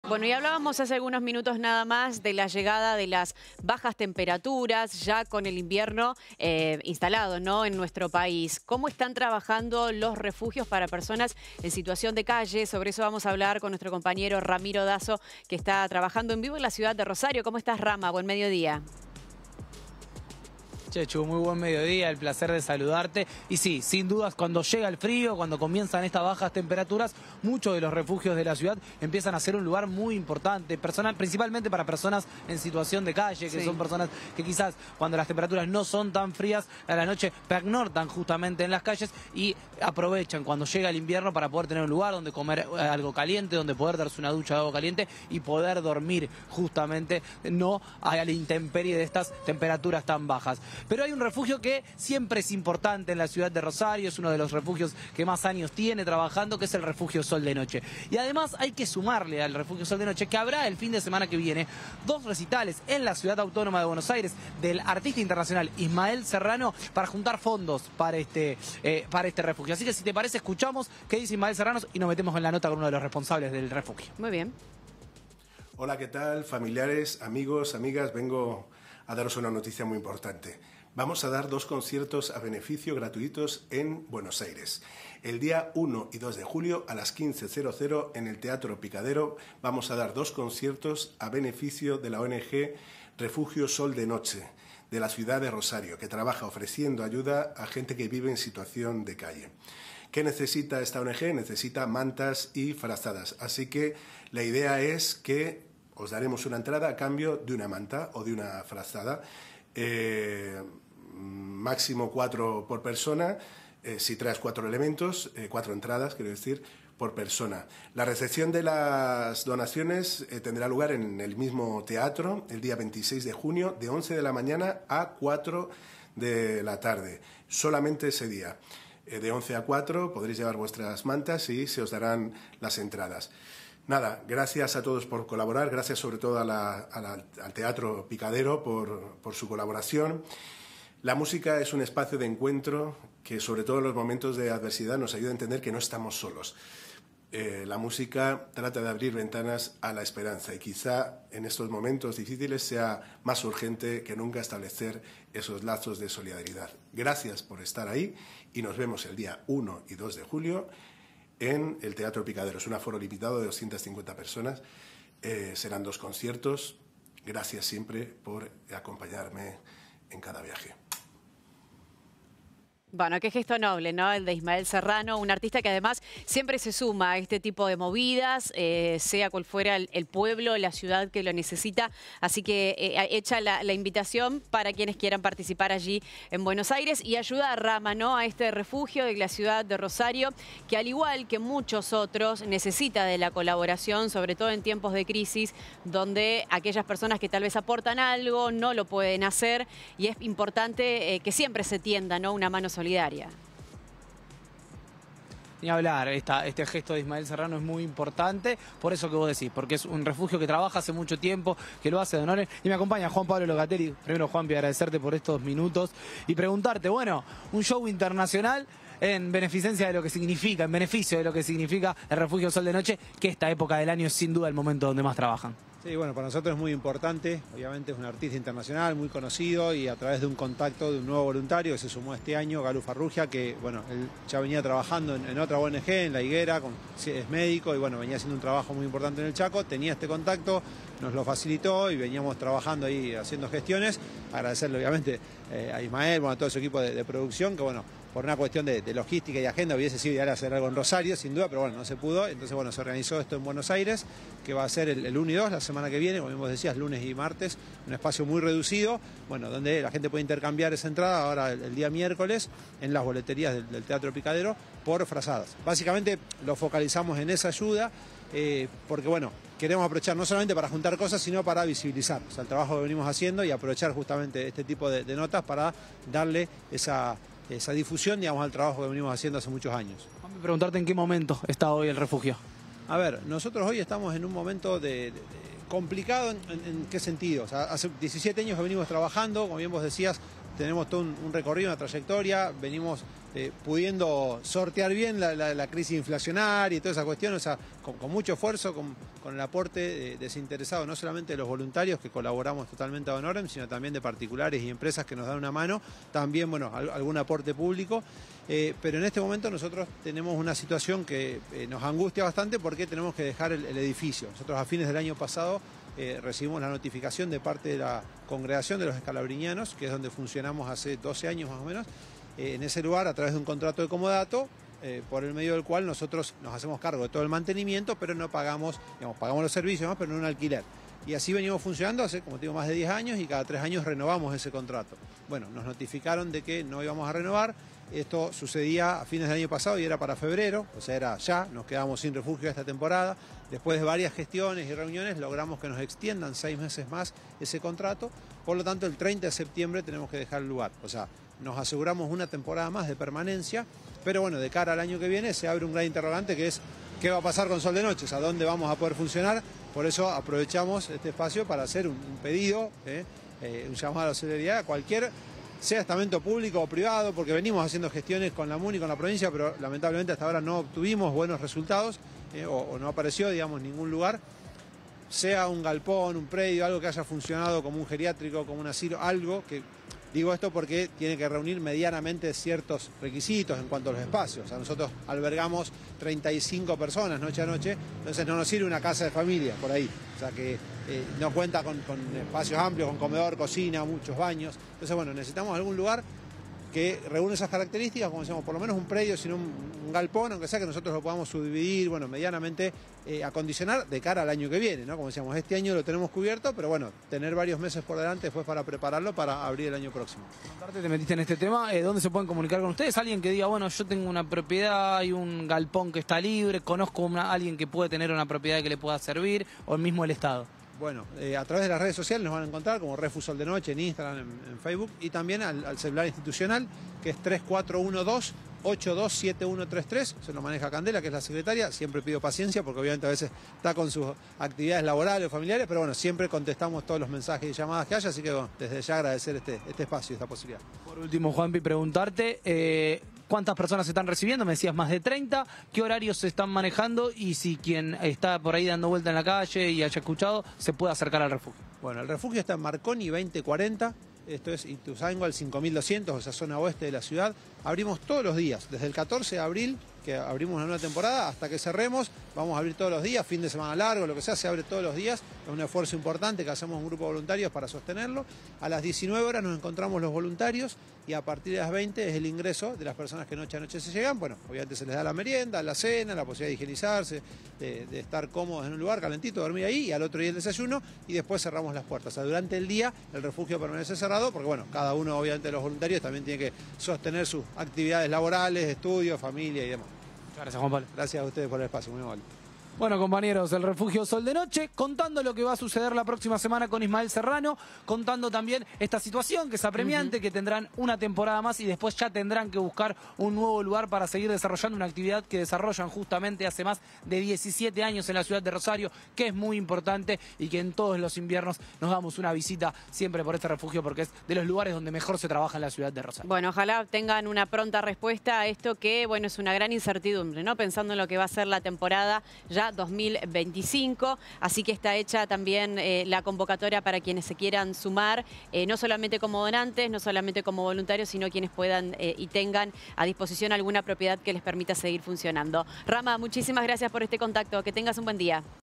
BM | Refugio Sol de Noche: Ismael Serrano dará dos shows gratis a beneficio de la ONG
Los espectáculos se realizarán el 1 y 2 de julio en el Teatro Picadero. Las entradas podrán ser canjeadas por mantas y frazadas, que se destinarán a la asociación civil ubicada en Marconi 2040.